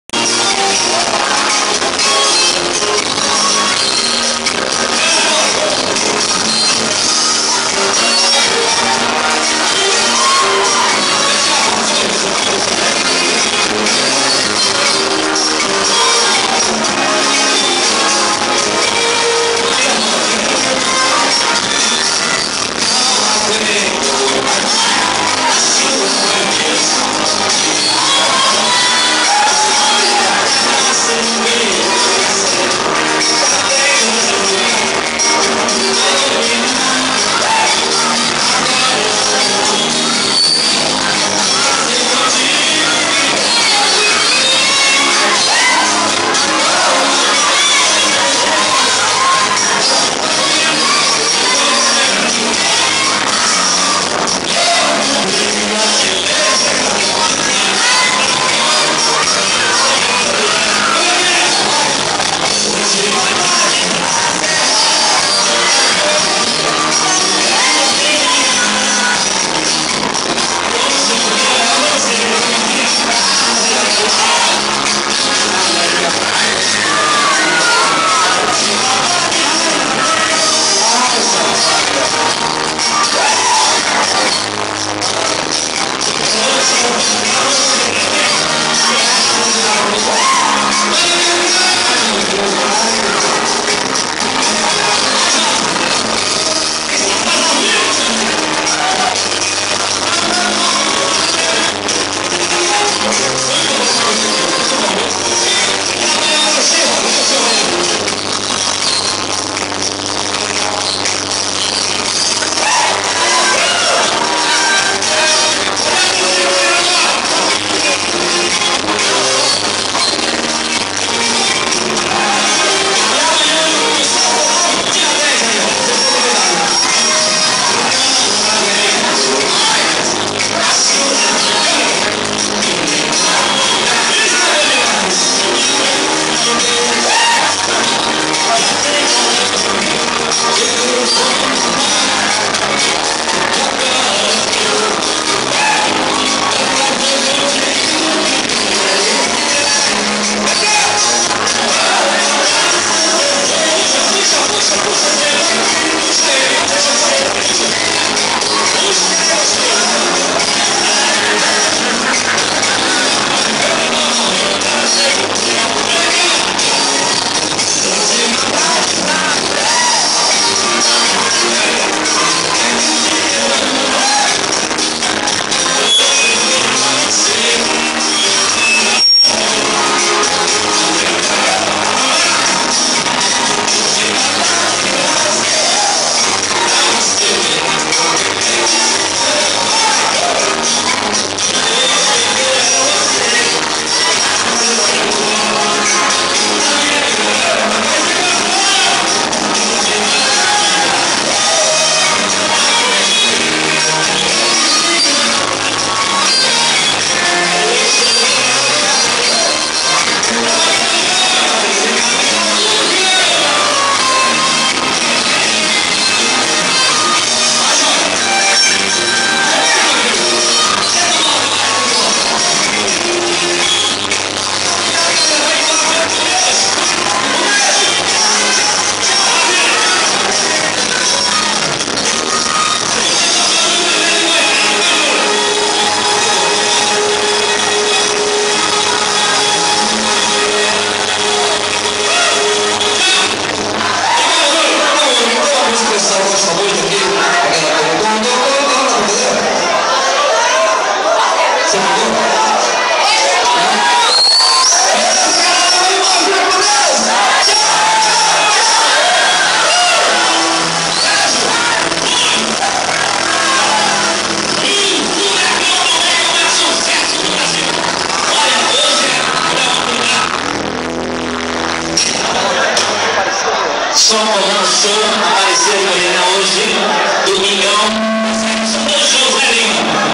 Júlio Nascimento e Américo Lopes cantando juntos em Goiânia
Dia muito bom esse